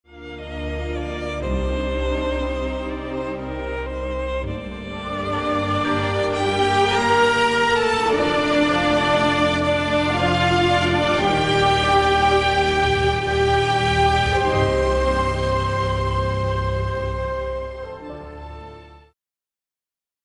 SPOT -Ajutati-l pe Dumitru
Are nevoie de 200.000 de euro pentru un transplant de inima